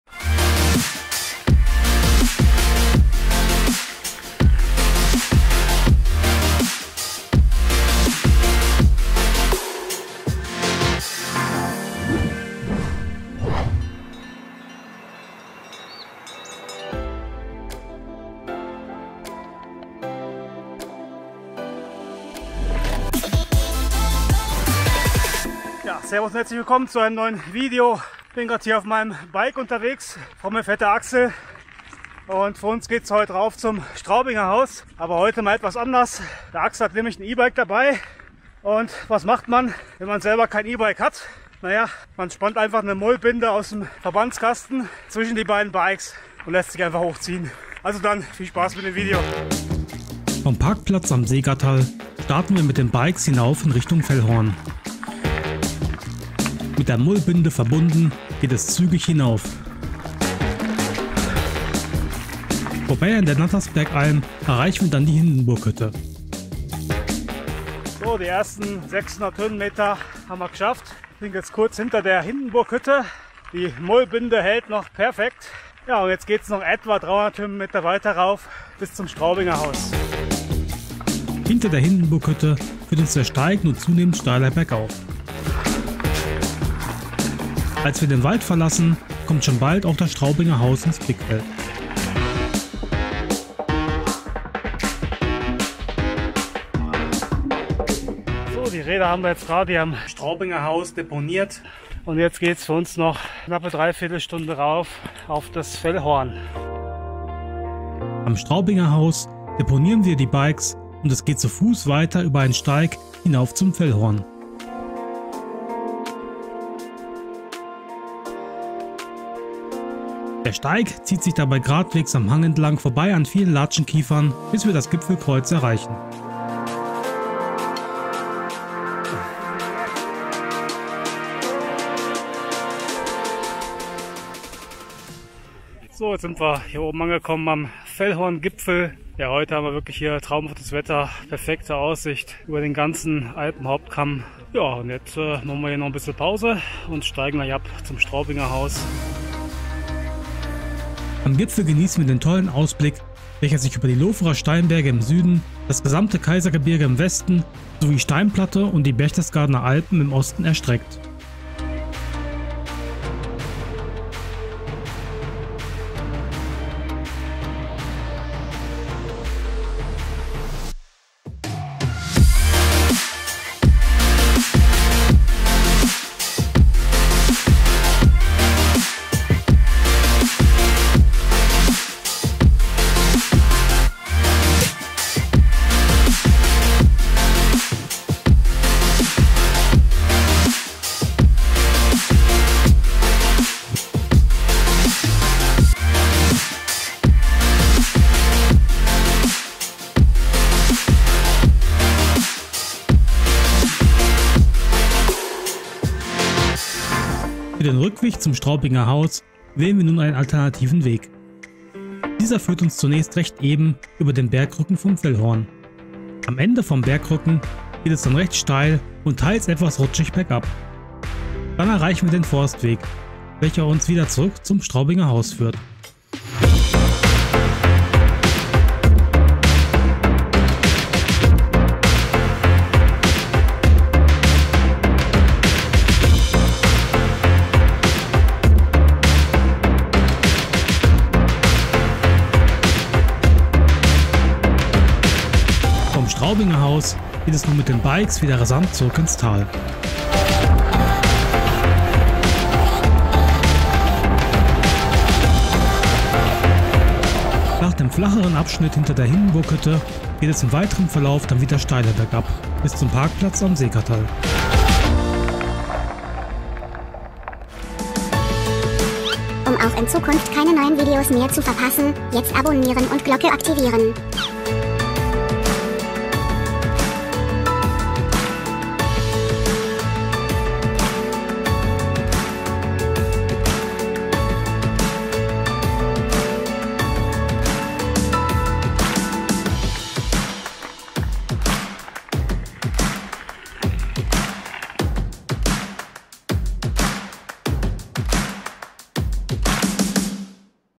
Bike&Hike zum Fellhorn | Chiemgauer Alpen
Mit dem Bike starten wir vom Seegatterl hinauf auf das Fellhorn. Den Geschwindigkeitsunterschied zwischen Bio- und e-Bike gleichen wir dabei auf eine etwas unkonventionelle Weise aus ;)

► Headshaker Merch @ Sauba Bleim:
►► Unterstütze Naturschutzprojekte in den Alpen und kaufe nachhaltig produzierte Textilien mit dem coolen Headshaker Logo bei Sauba Bleim